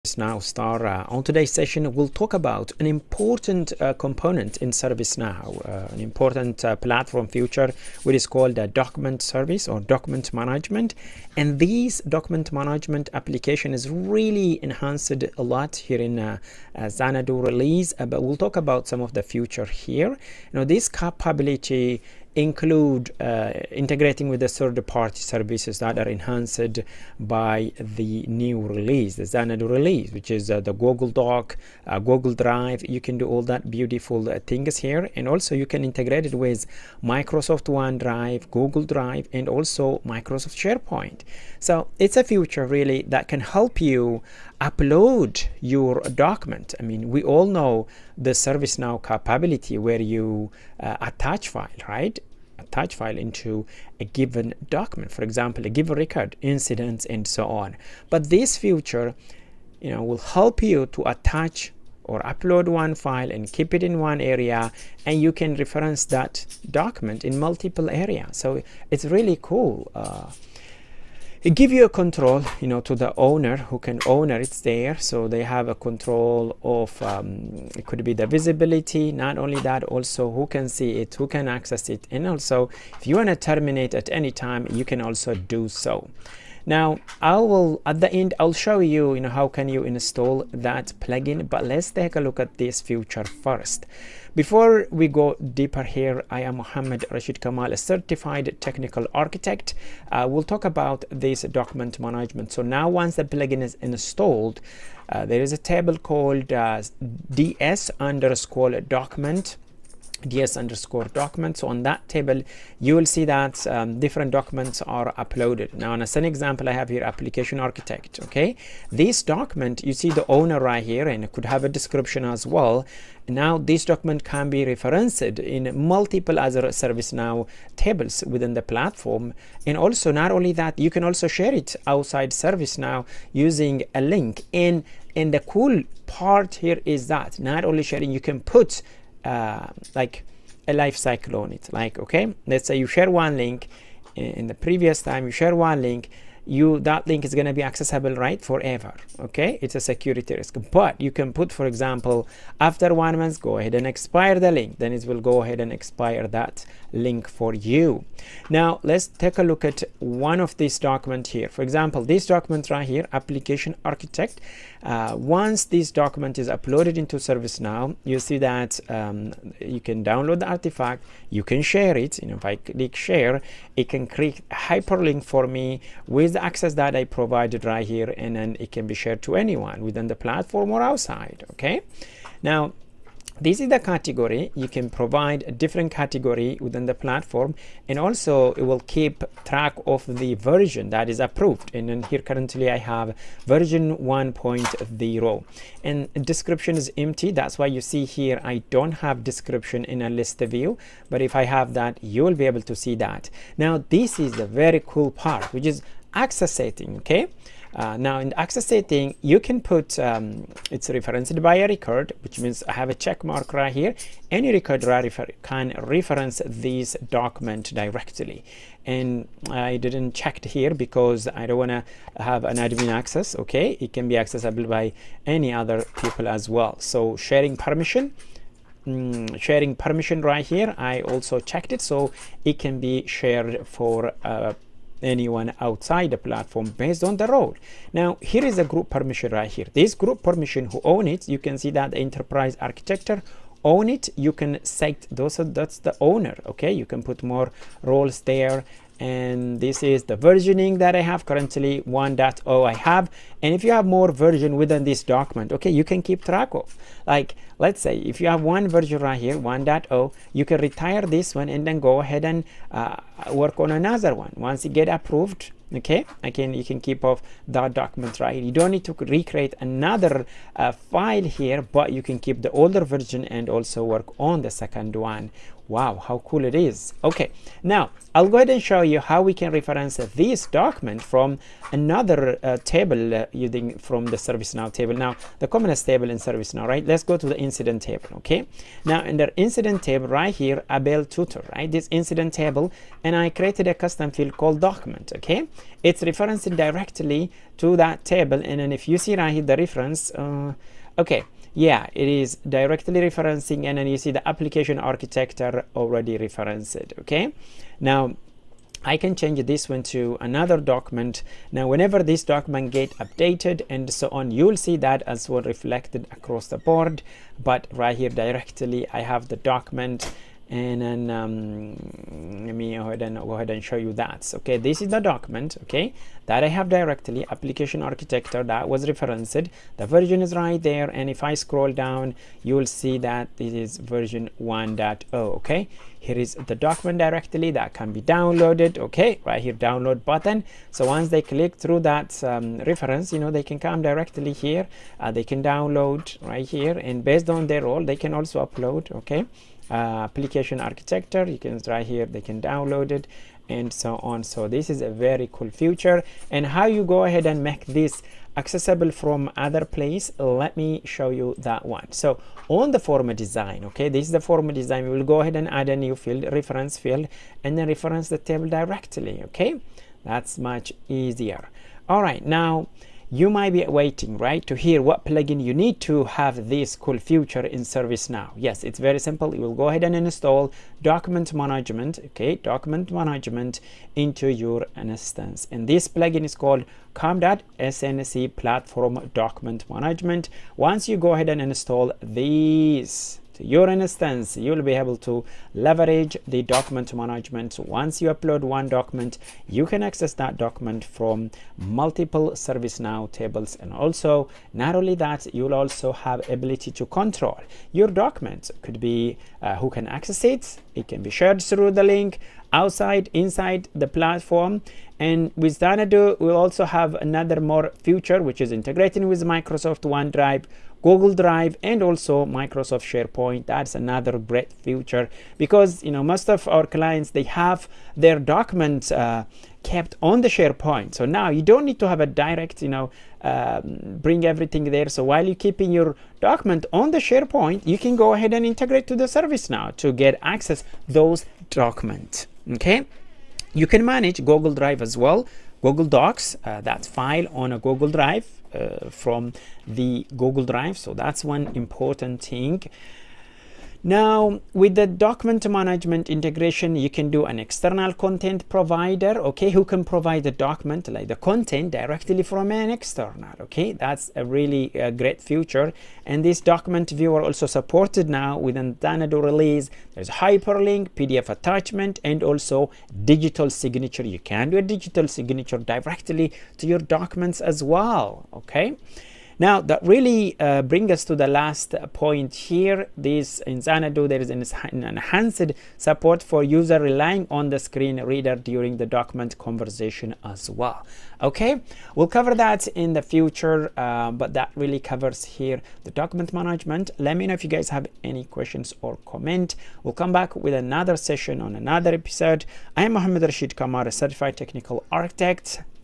Master ServiceNow Document Management: All-in-One Guide 2025
Learn how to streamline your document management with ServiceNow. This video covers key features, best practices, and real-world examples to help you improve efficiency and productivity

0:00 Intro
0:11 Document management Xanadu release features
1:42 Demo accessing document management modules, document lifecycle
3:28 ServiceNow document management access control
4:12 ServiceNow document management version control
4:40 Demo on How to reference document from another table in ServiceNow
6:13 How to install the ServiceNow document management plugin
8:21 Closing